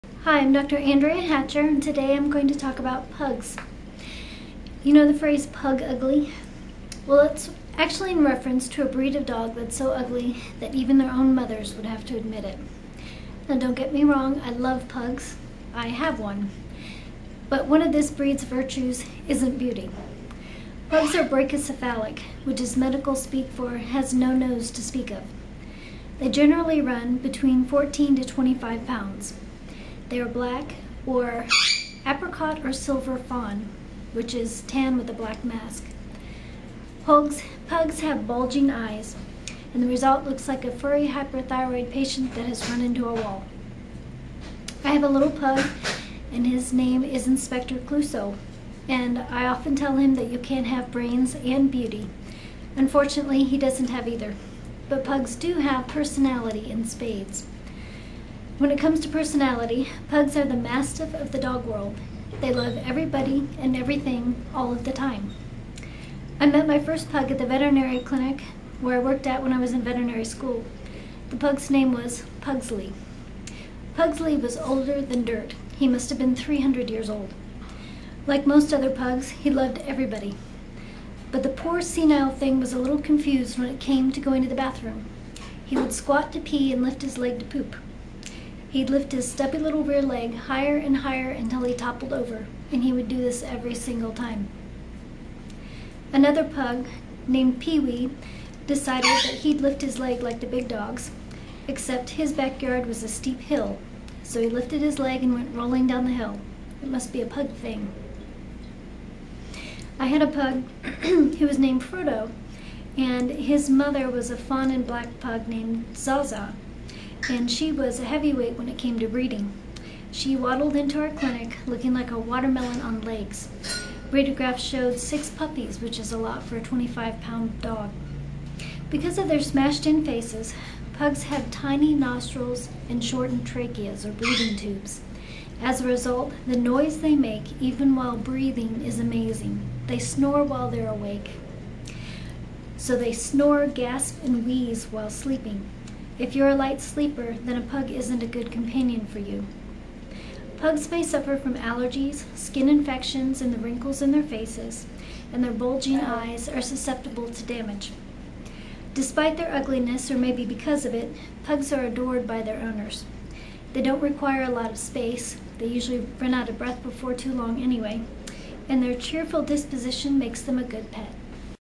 Pugs
This is an informational video about Pugs.

Dr. Anndrea Hatcher

Olive Branch Parke Veterinary Clinic